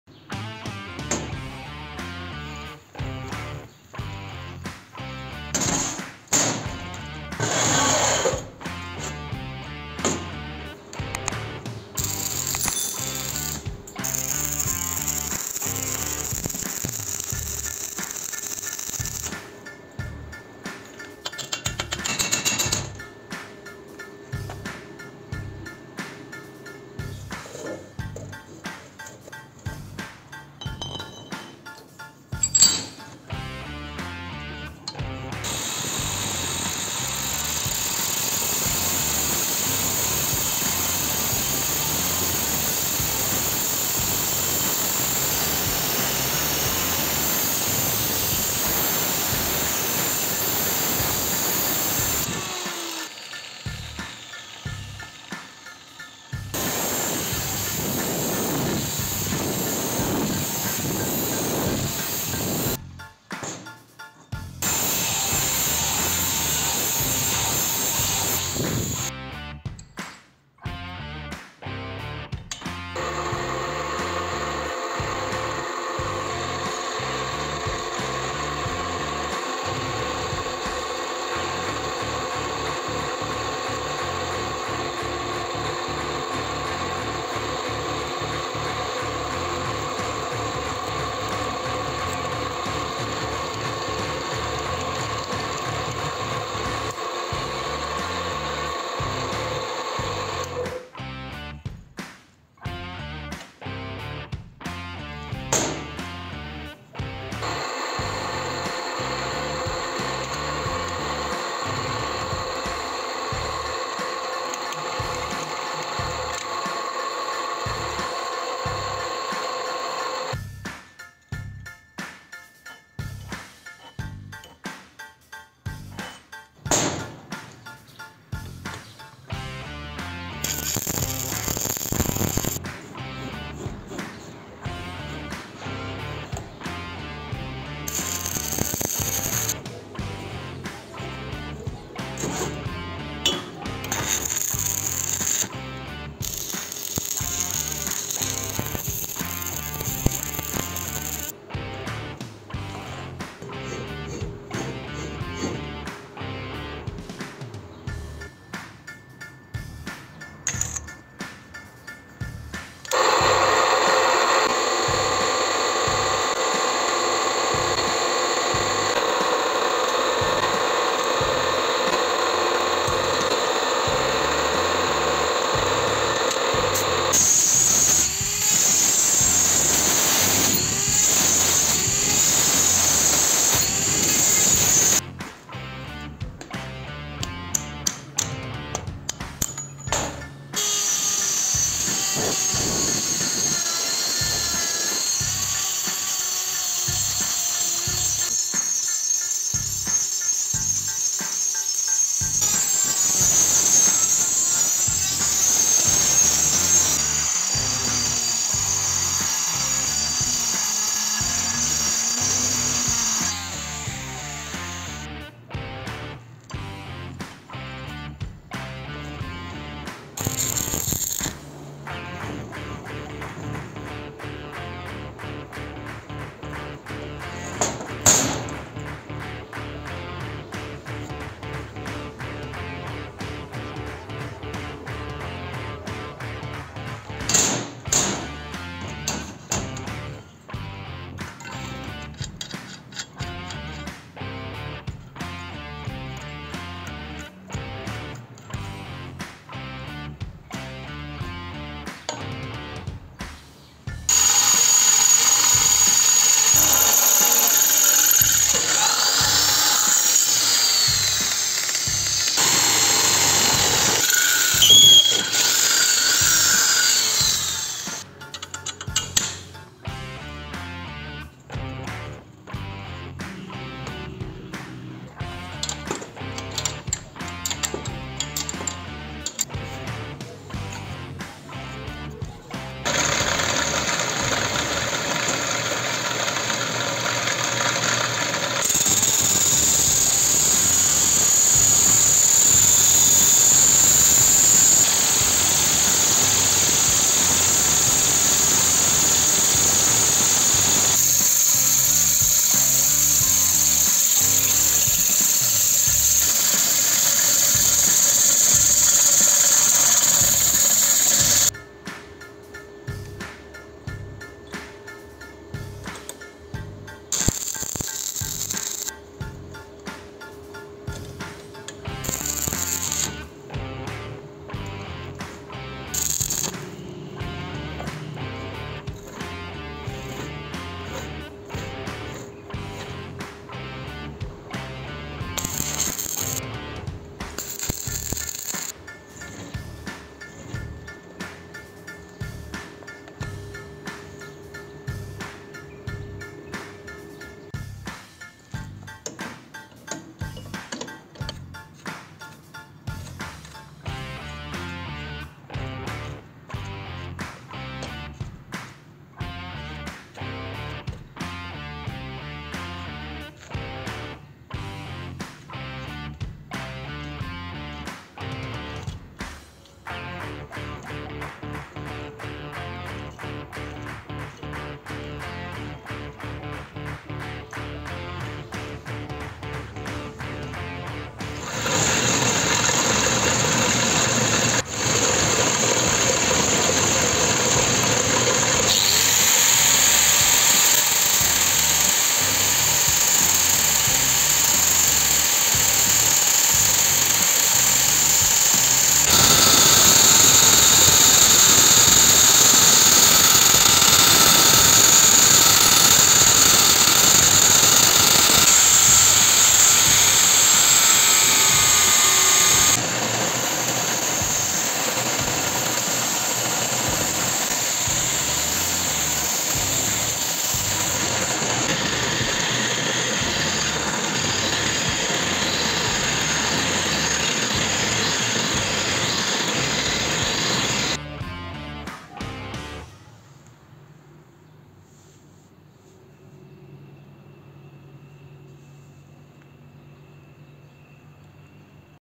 I have always needed this handmade tool in my workshop
Welcome to INNOVATION TIME.💐
In this video, I have made an amazing useful DIY tool.
I know it should have been made better than this. It was better to use two bearings with one tube inside them. But anyway, this is an idea that can do some of the work for a lathe for me
Make it easily 😉